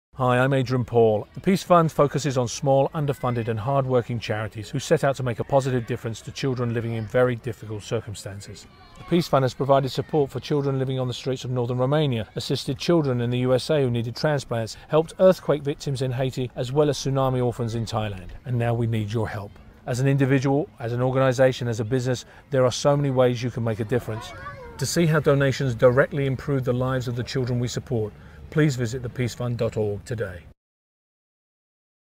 PEACE Fund PSA 1
Adrian Paul discusses the mission of The PEACE Fund.  thepeacefund.org